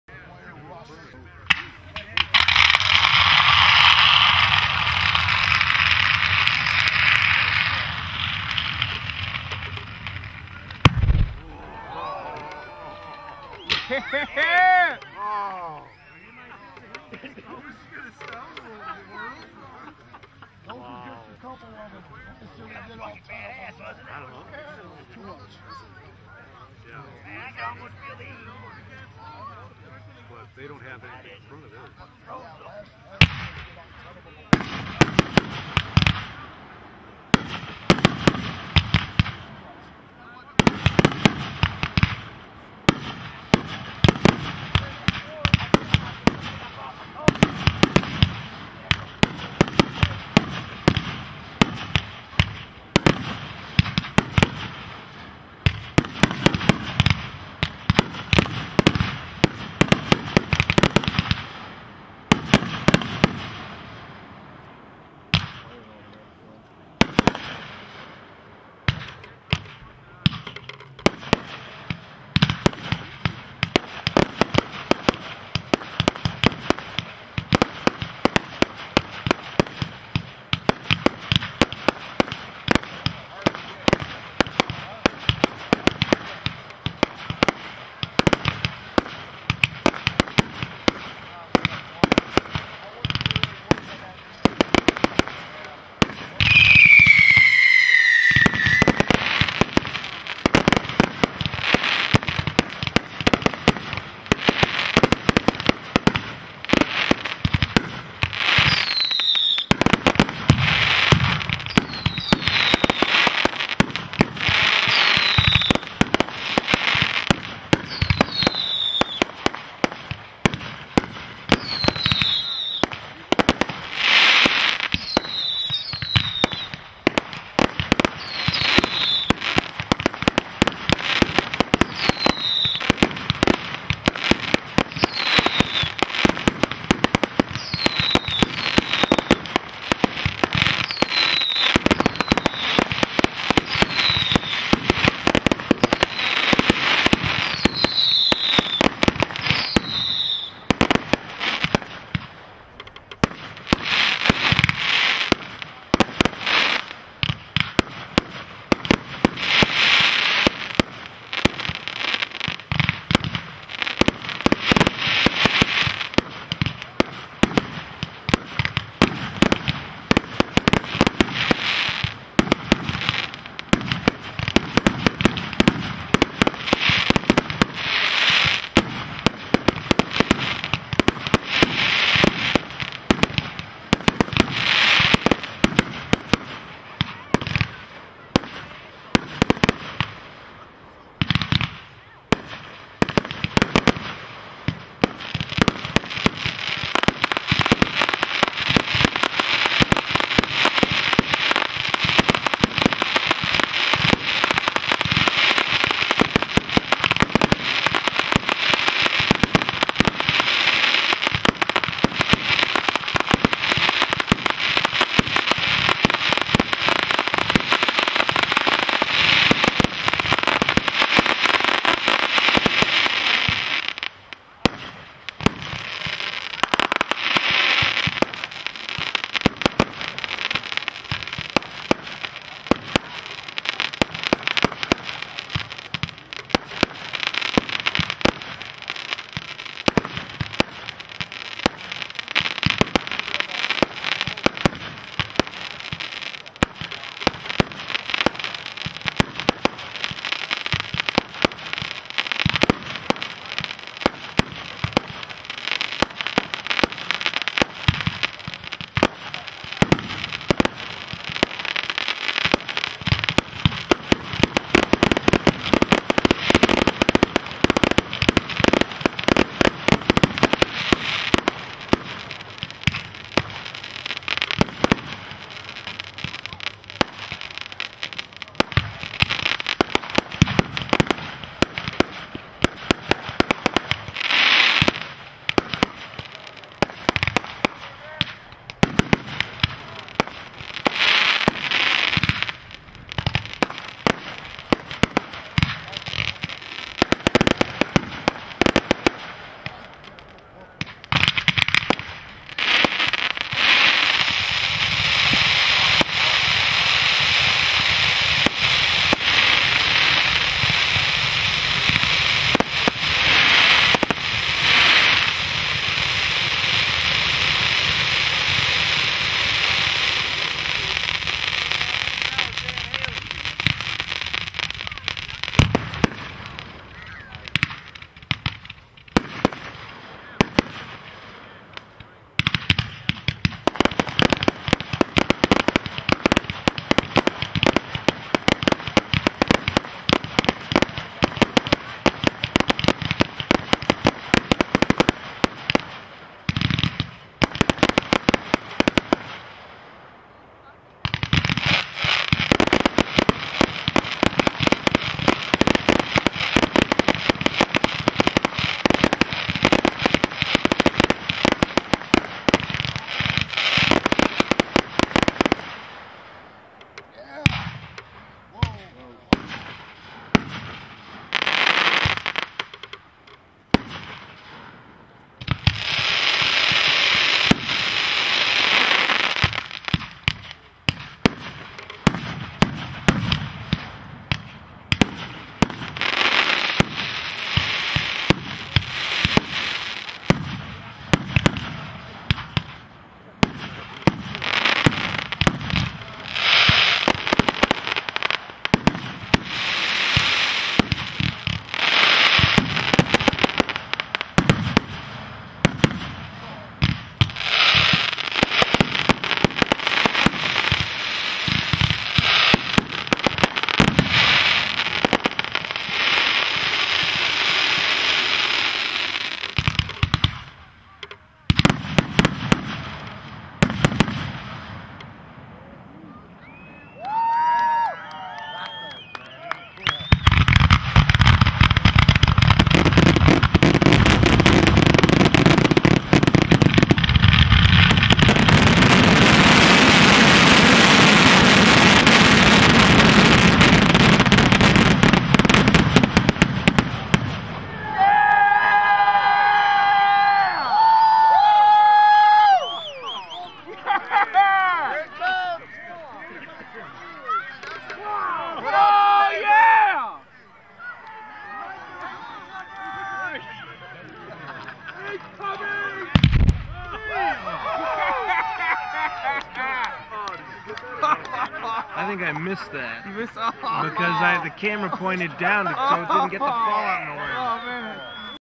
GLASS 2012 - Tri-Star
Tri-Star team show for GLASS 2012
Lead Shooter:  finale jim
Crew:  deano the pyro, TheJester, Kate, Wintermute
Firing System:  ODA50Q